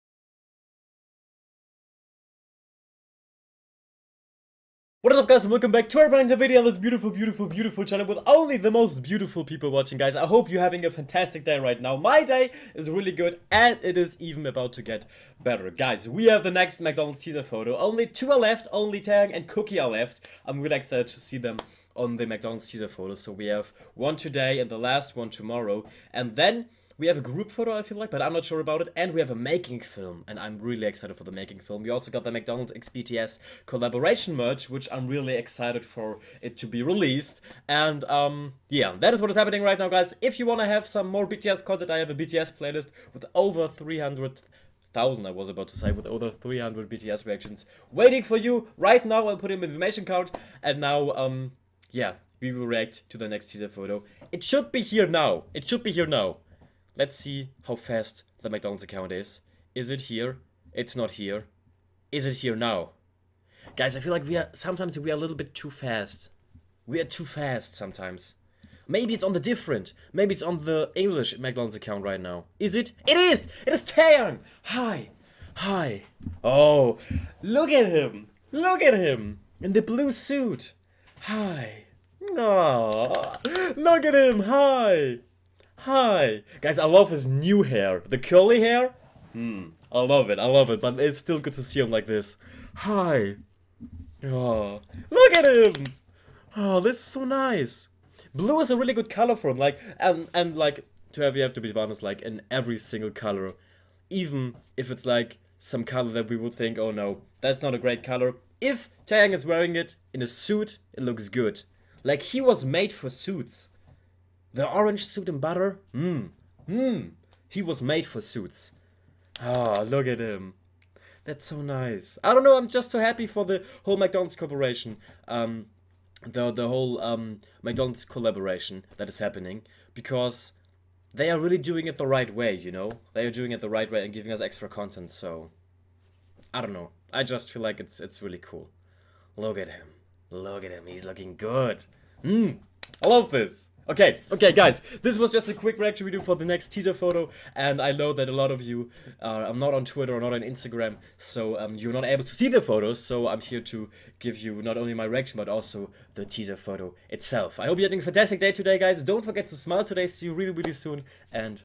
LOOK AT HIM! 🥰😆 BTS X MCDONALDS TAEHYUNG TEASER PHOTO REACTION
Flo M.
Postfach 11 61
25442 Quickborn
Germany

Flo M.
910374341
Packstation 117
25451 Quickborn
Germany